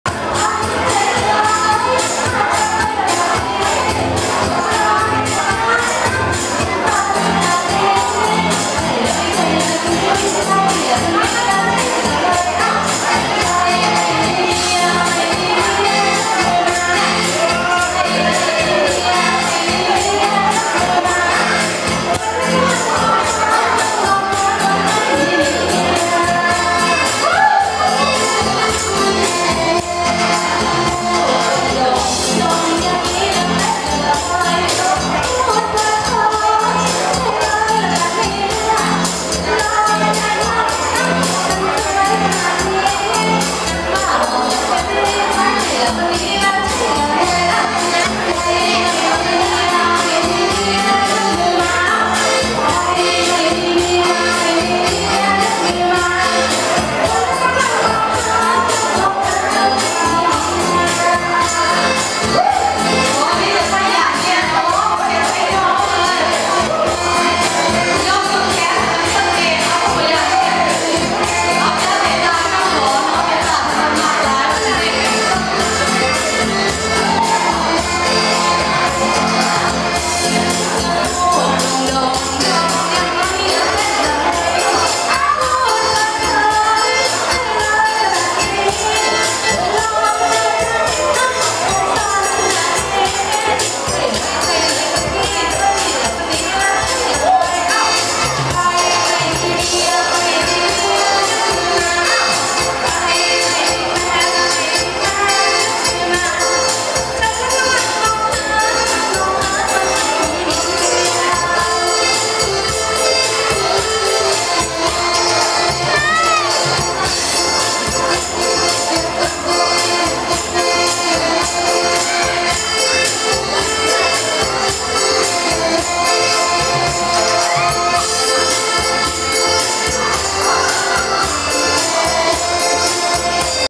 Chansamone Phetsomphong at "Wat Findlay" Fundraising Party 08/06/2011 (2)
Chansamone Phetsomphong and friends.
Leaders and members of Wat Findlay Ohio held a fundraising party (Free food; $10 admission each and was entered for a prize drawing. Our team didn't win any!). Professional singer "Nam Pheung MouangChanh" was there from beginning until closing time entertaining the guests. We all had fun despite the heat in the hall.